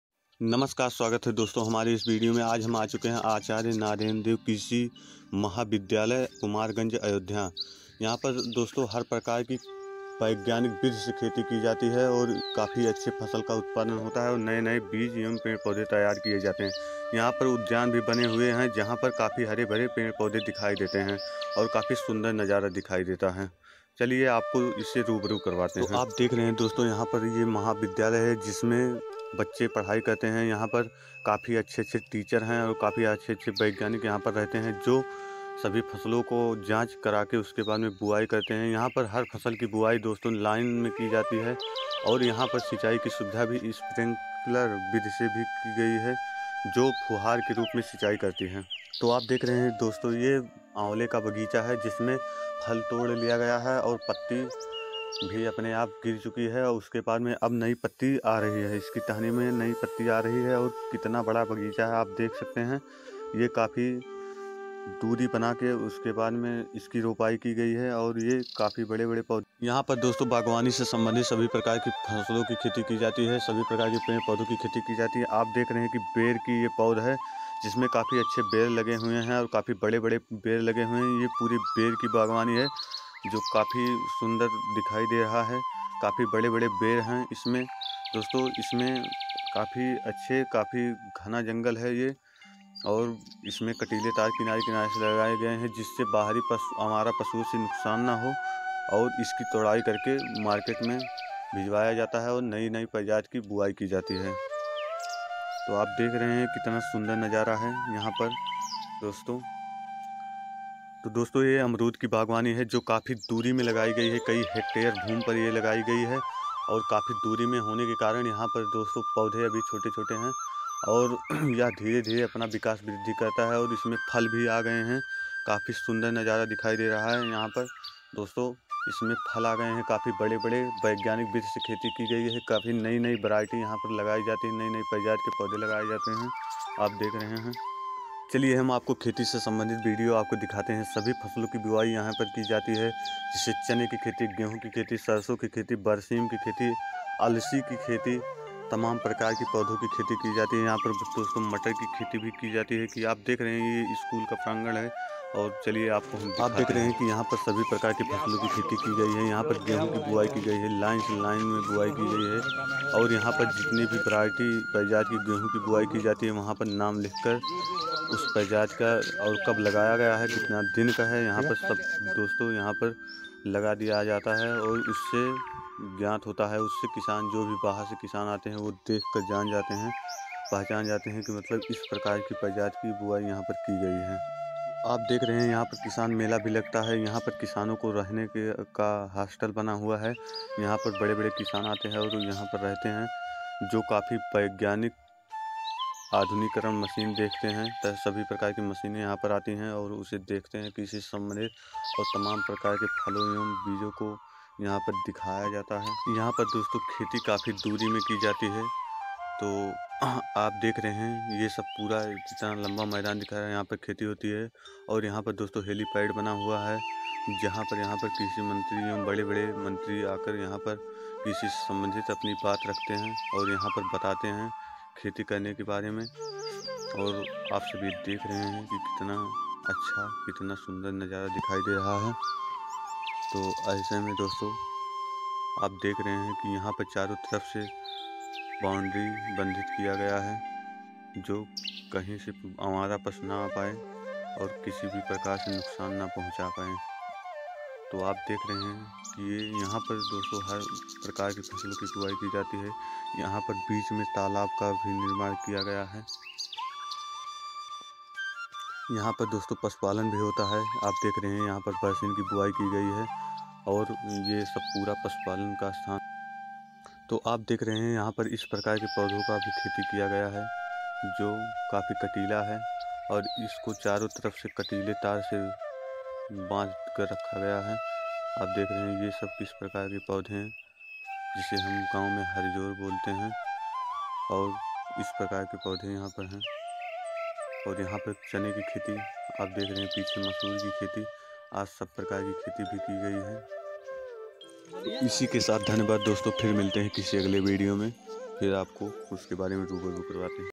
# Acharya Narendra Deva university Of Agriculture And Technology Kumarganj Ayodhya #
आचार्य नरेन्द्र देव कृषि महाविद्यालय कुमारगंज अयोध्या आप को इसके बारे में सटीक जानकारी दिया गया है